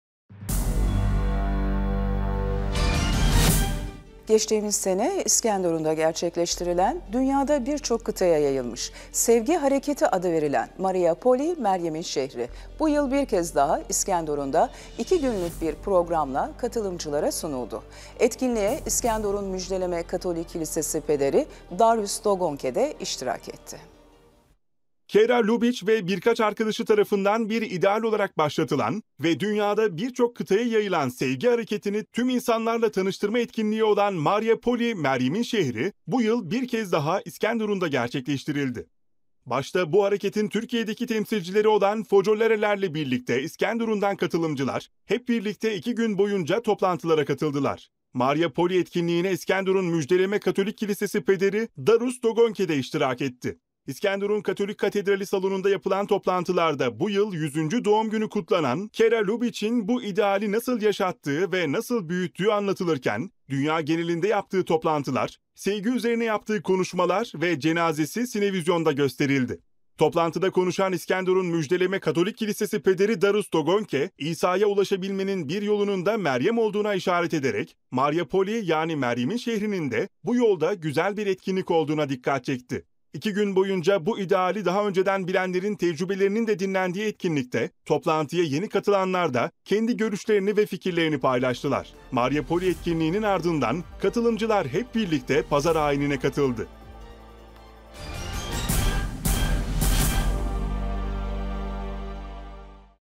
Mariapoli, Yeniden İskenderun’da | 14.02.2020
hiara Lubich ve birkaç arkadaşı tarafından bir ideal olarak başlatılan ve dünyada birçok kıtaya yayılan ‘Sevgi’ hareketini, tüm insanlarla tanıştırma etkinliği olan ‘Mariapoli’ (Meryem’in Şehri), bu yıl bir kez daha İskenderun’da gerçekleştirildi.
Başta bu hareketin Türkiye’deki temsilcileri olan Focolare’ler ile birlikte İskenderun’dan katılımcılar, hep birlikte iki gün boyunca toplantılara katıldılar. Mariapoli Etkinliğine, İskenderun Müjdeleme Katolik Kilisesi Pederi Dariusz Dogondke de iştirak etti.
İskenderun Katolik Katedrali salonunda yapılan toplantılarda, bu yıl 100. Doğum Günü kutlanan Chiara Lubich’in bu ideali nasıl yaşattığı ve nasıl büyüttüğü anlatılırken, dünya genelinde yaptığı toplantılar, ‘sevgi’ üzerine yaptığı konuşmalar ve cenazesi sinevizyon şeklinde katılımcılara izlettirildi.
Toplantıda konuşan İskenderun Müjdeleme Katolik Kilisesi Pederi Dariusz Dogondke, İsa’ya ulaşabilmenin bir yolunun da Meryem olduğuna işaret ederek, ‘Mariapoli’ (Meryem’in Şehri)’nin de bu yolda güzel bir etkinlik olduğuna dikkat çekti.
İki gün boyunca bu ideali daha önceden bilenlerin tecrübelerinin de dinlendiği etkinlikte, toplantıya yeni katılanlar da kendi görüşlerini ve fikirlerini paylaştılar. Katılımcılar paylaşımların yanı sıra birlikte yemek de yediler. Chiara Lubich ve birkaç arkadaşı tarafından Birinci Dünya Savaşı döneminde bir ideal olarak başlatılan ve dünyada birçok kıtaya yayılan Mariapoli etkinliğinin İskenderun ayağında, katılımcılar hep birlikte Pazar ayinine katıldı. (SAT-7 TÜRK / İlyas Edip TERBİYELİ-İskenderun)